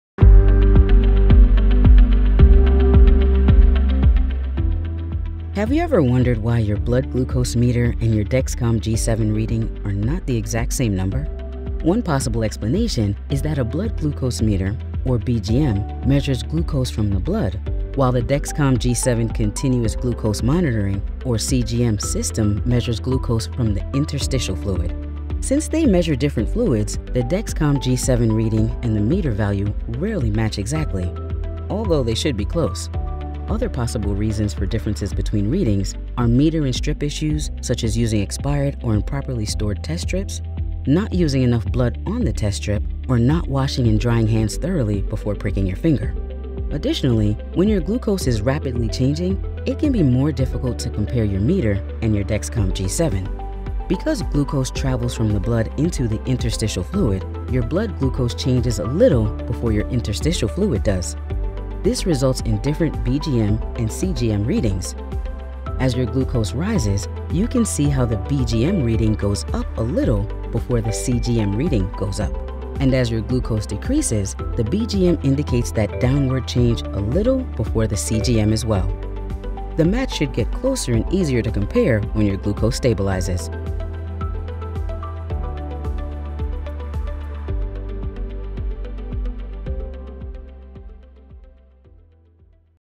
Dexcom G7 – Accuracy
Have you ever wondered why your blood glucose meter value and your Dexcom CGM reading are not the exact same number?

Watch this video to learn more about the accuracy of your Dexcom CGM and the difference between blood glucose monitor and CGM readings.

LBL-1001617 Rev001